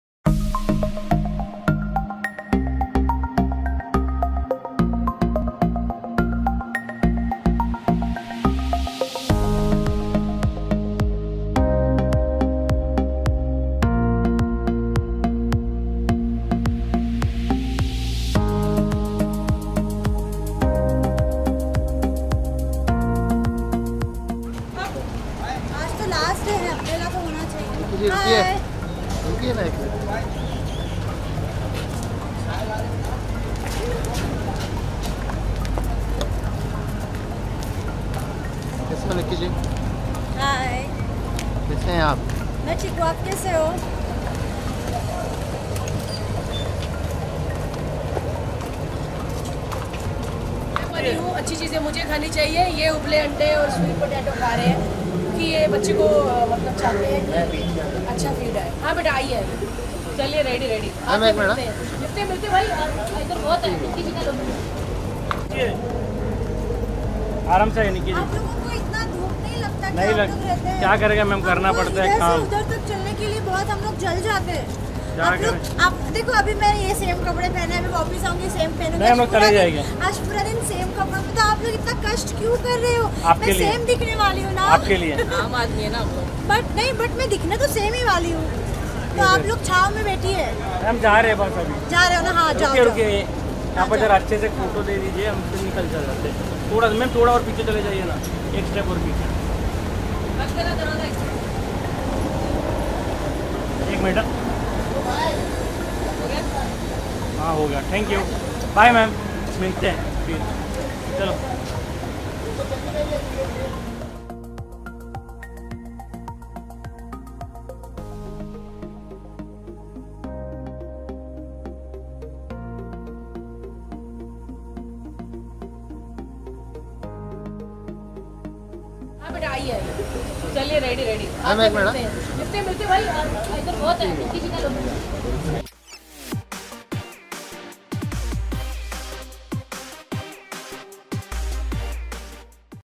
Nikki Tamboli, Bharti Singh & Haarsh Limbachiya At ‘The Khatra Khatra Show’ Set | BoogleBollywood
Copyrights@BoogleBollywood